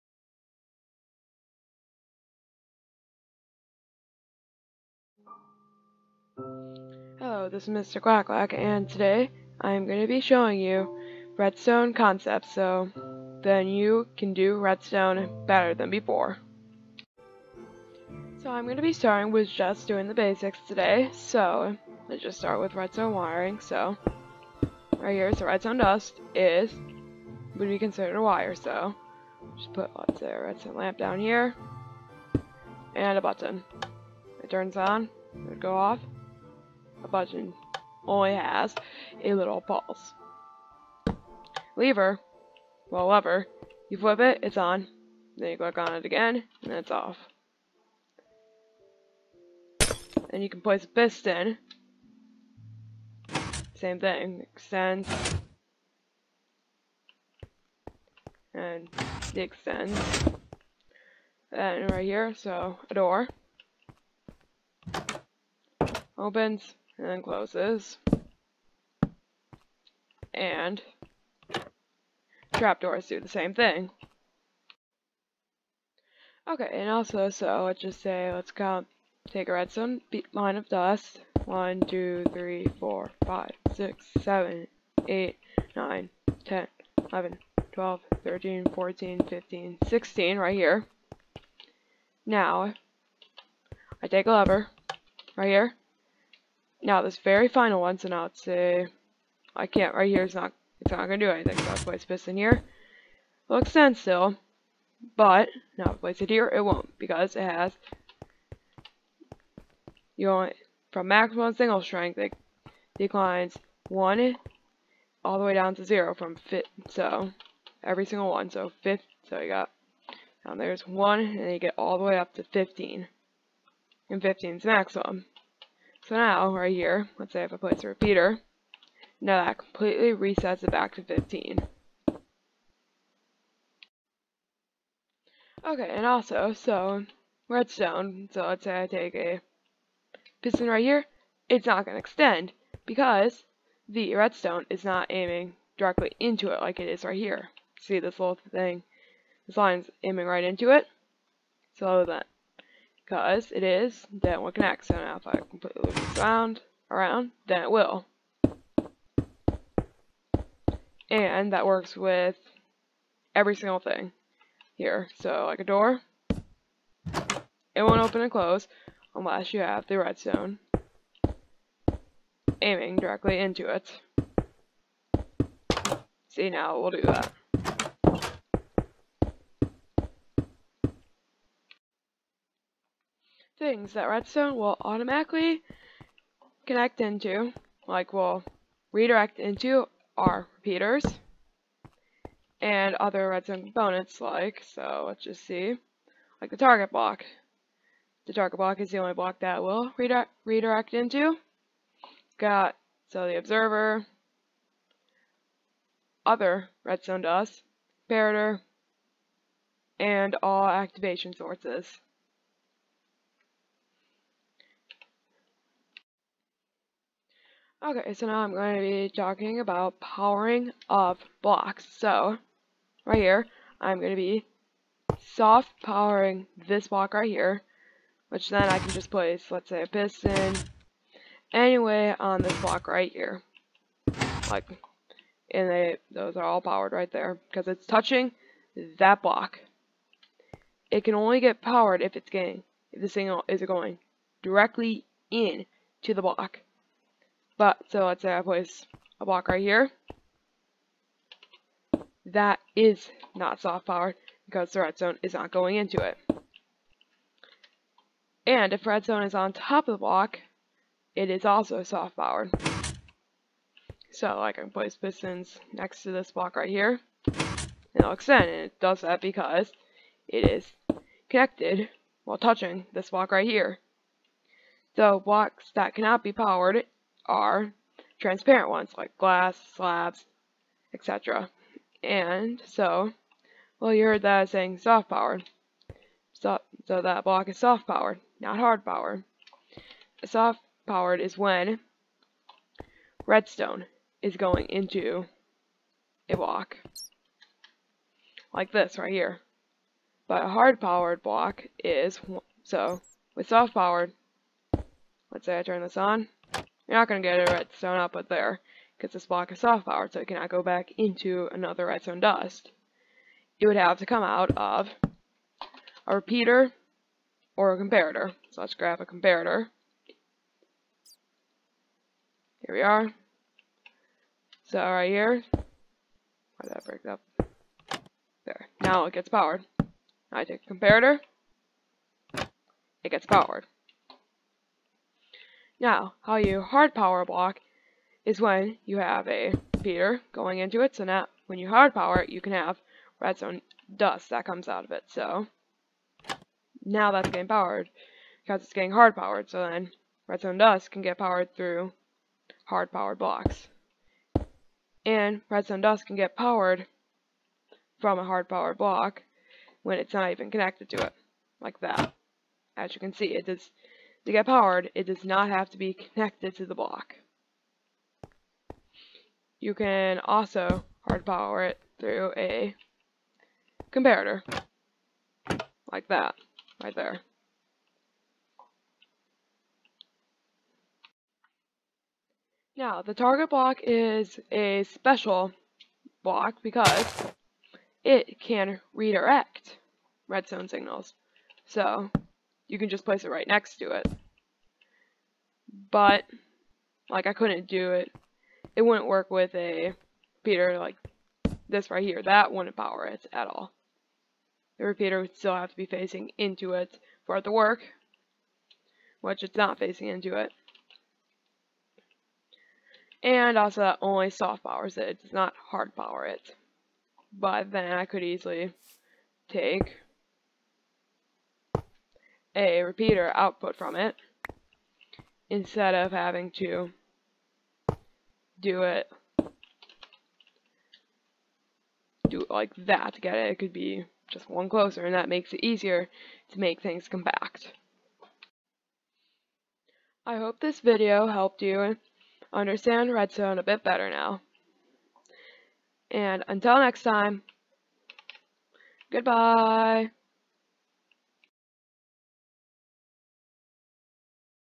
Redstone Concepts: The Basics
Today I show you the redstone basics. I hope this helps you in redstone.